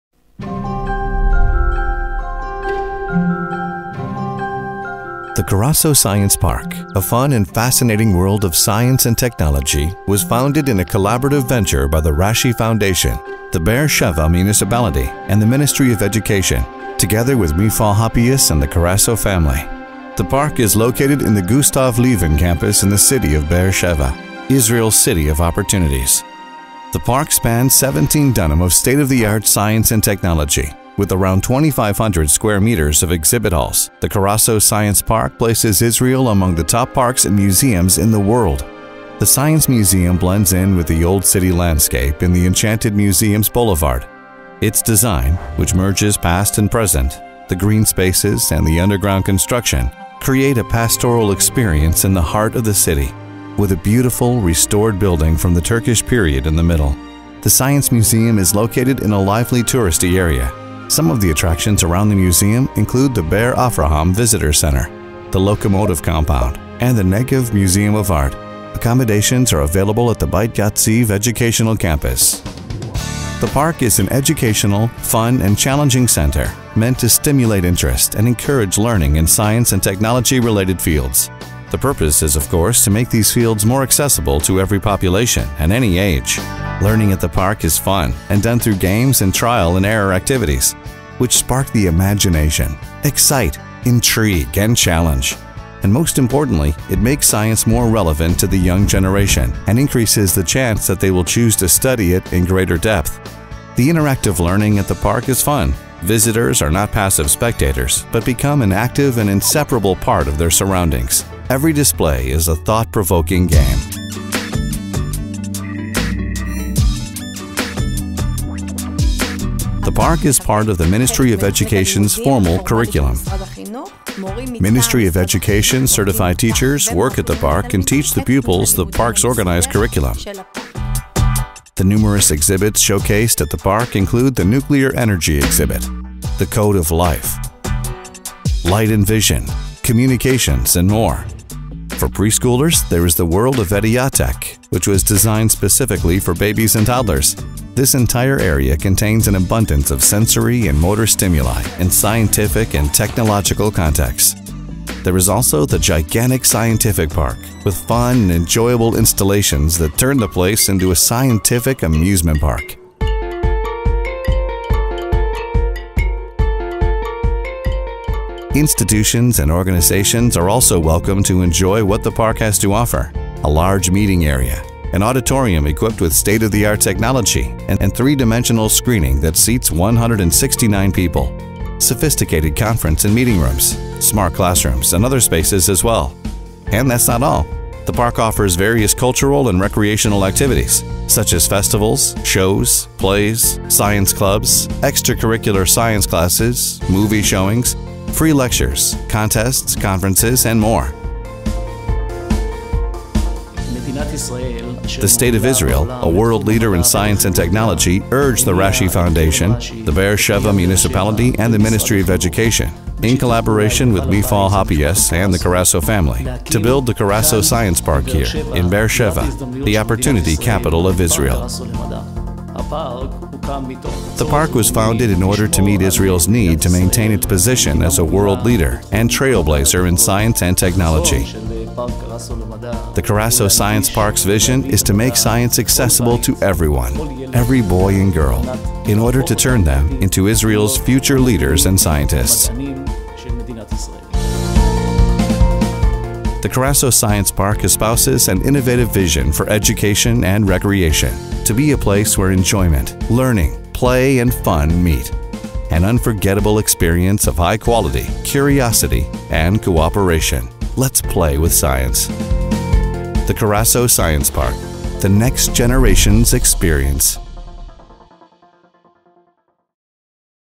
The Carasso Science Park - Be'er Sheva
The Carasso Science Park is spread over 4.25 acres and was established to give the people of Southern Israel, and the rest of the world, exposure to Israel’s scientific and technological innovations, discoveries and rich history, all under one roof. The Park was established on a site which includes historic buildings from the Ottoman Empire and the British Mandate periods. In addition to restoring these vital pieces of history, cutting-edge facilities were also established making the site one of the most unique and attractive in the world today. This is the largest Science Park in Israel, and acts as a magnet for new tourism in the Negev, helping to improve the economic growth in the region. The park was designed to inspire, intrigue, educate and motivate people of all ages, with emphasis on schoolchildren, understanding that one must foster leadership in order for it to happen.